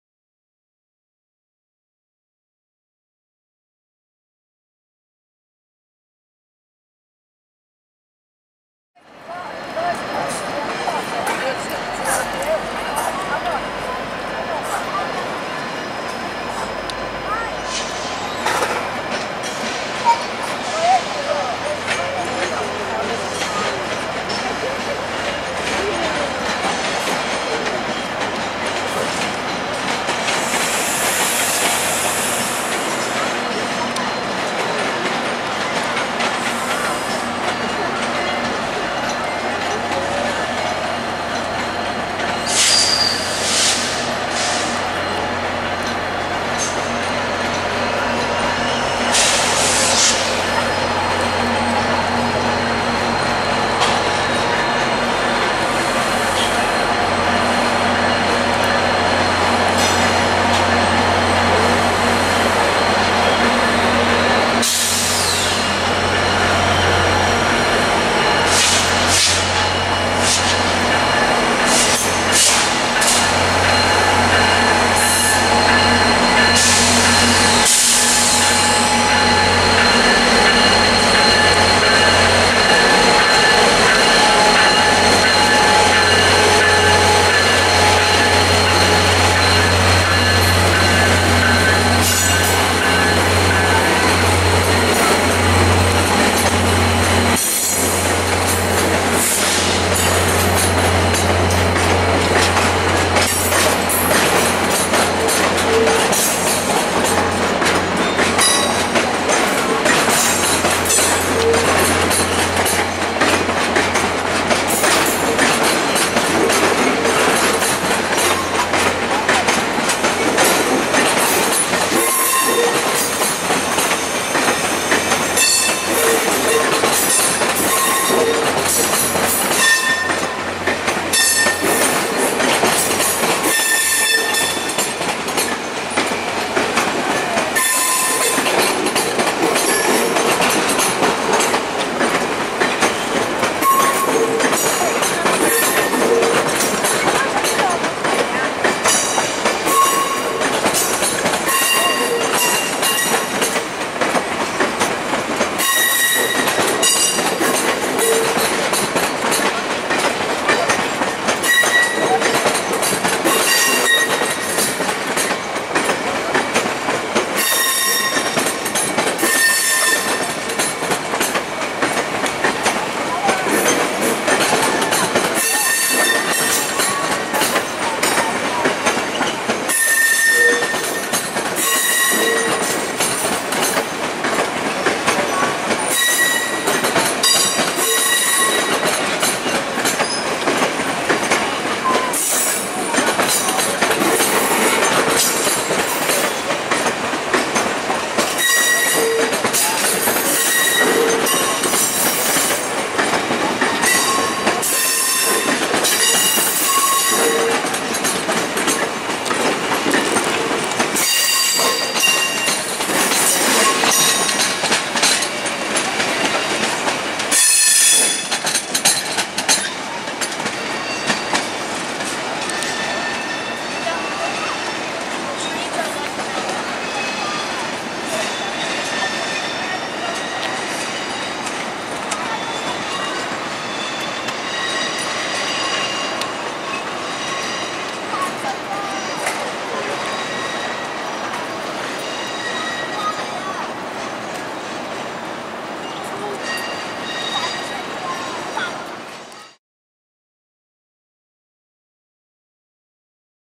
The Canadian Departing Toronto's Union Station July 21st, 2024
VIA Rail Canada's flagship train, The Canadian (Train #1) is seen departing Toronto's Union Station on the morning of July 21st, 2024 bound for Vancouver, British Columbia.

The train is powered by two F40PH-2 units, #6440 and #6408, has one HEP-I baggage car, two HEP-I coaches, three Skylines, two dining cars (Princess and Palliser), nine Manor sleepers, two Chateau/Prestige sleepers, and Glacier Park bringing up the rear.